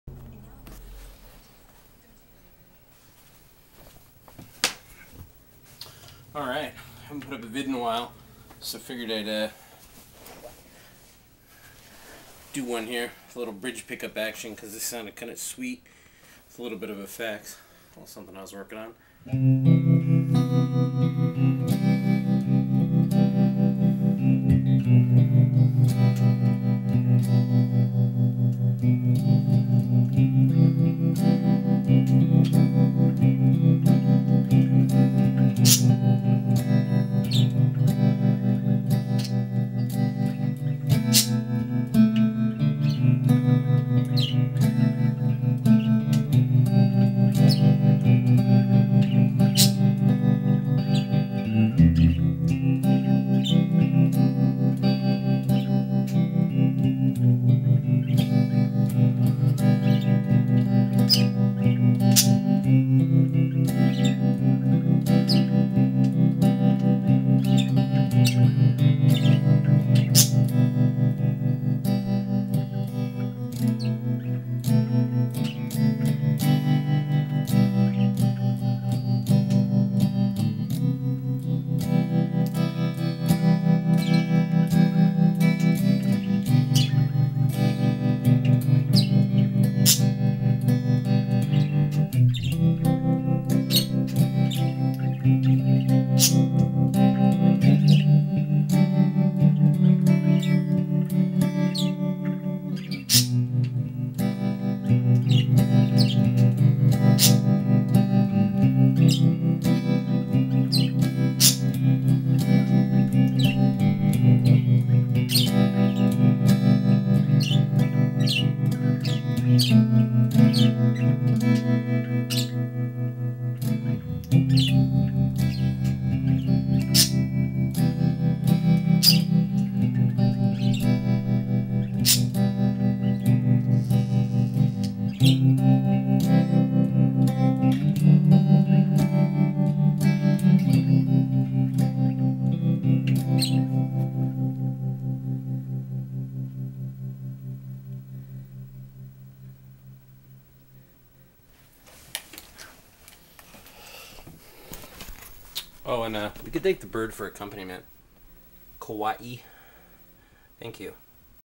A Custom Classic Strat, LoneStar Special, RP250, a Bird, and me
New chord progression I have been working on here that sounded nice with Tremolo through a Digitech RP250. The TV is not on in the background as usual, but the bird does offer unwelcome participation. I live in a world of sound and cannot escape. In closing, I love maple necks on guitars. The one on this one is highly flamed and glossed.